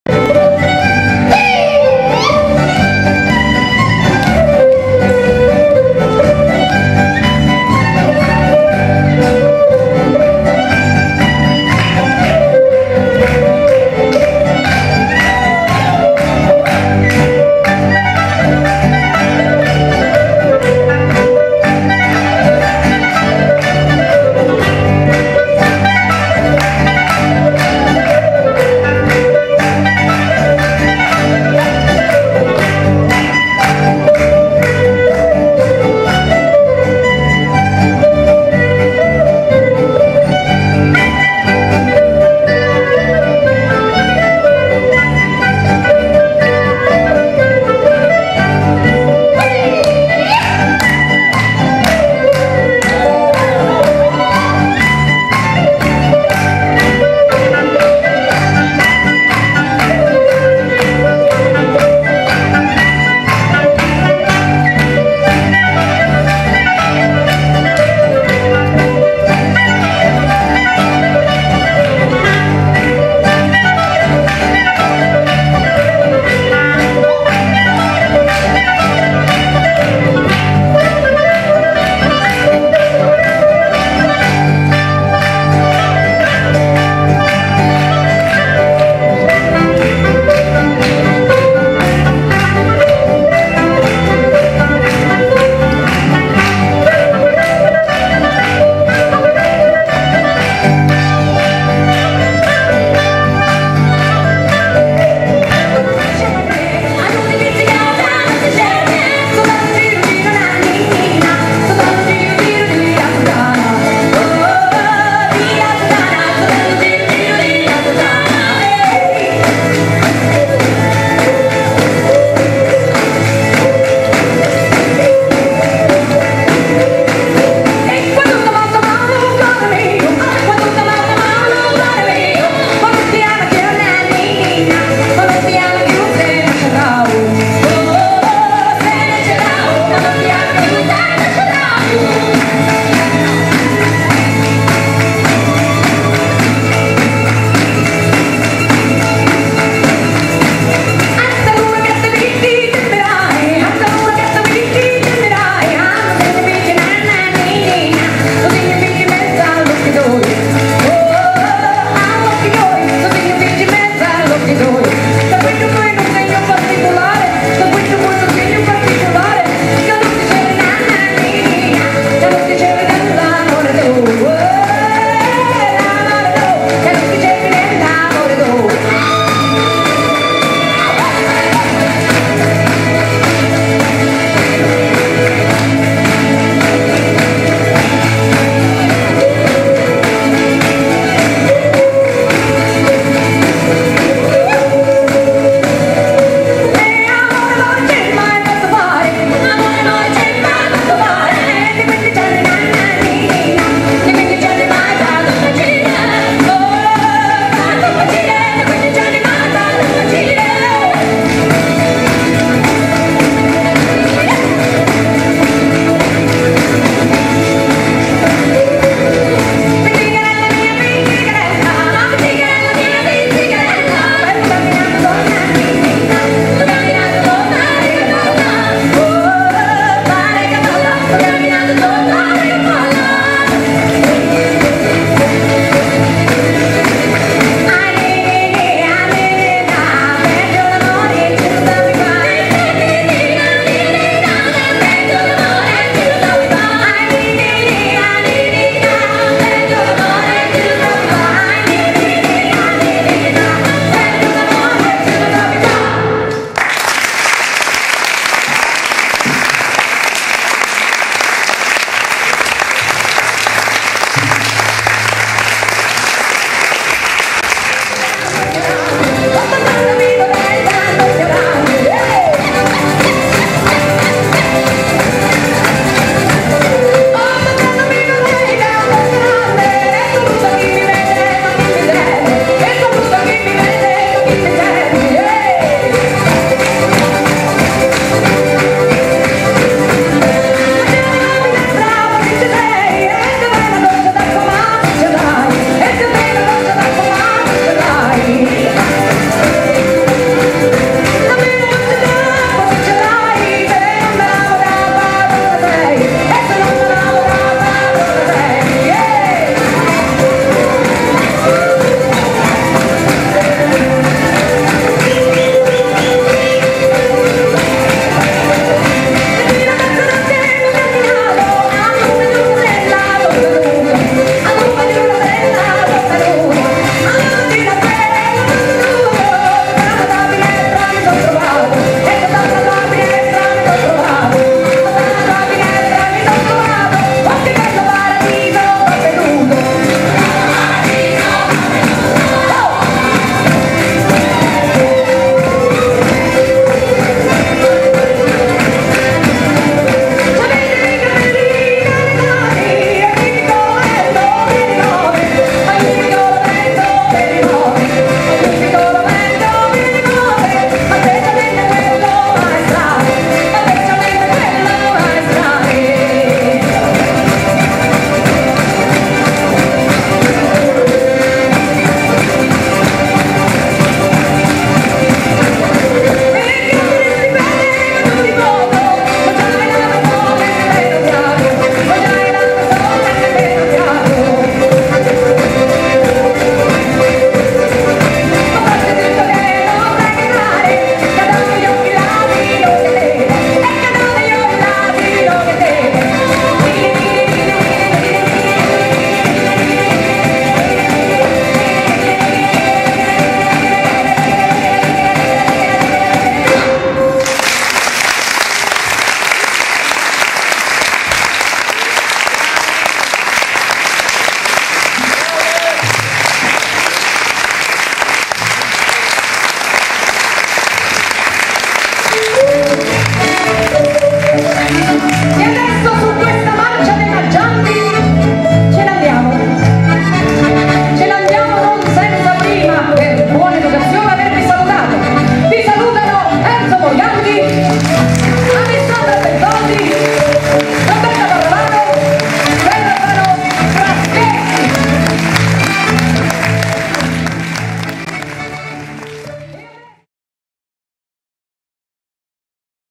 Maggio, Maggio è Capitano - 2007 - parte 3^
"MAGGIO, MAGGIO E' CAPITANO"
28, 29, 30 maggio 2007 Teatro Vascello Roma
con LUCILLA GALEAZZI, L'albero Del Canto, Poliandro, Massimo Nardi, Salvatore Zampataro, Claudio Merico , Antonio Ramous, Umberto Papadia
con la partecipazione di Nando Citarella
Ospiti Ambrogio Sparagna, Rita Marcotulli, Tetes de Bois
Musiche Lucilla Galeazzi
Arrangiamenti Massimo Nardi e Lucilla Galeazzi
Coreografie Alessandra Bertoldi
Danzatori Alessandra Bertoldi, Stefano Fraschetti, Enzo Morganti, Roberta Parravano
Ideato e realizzato da LUCILLA GALEAZZI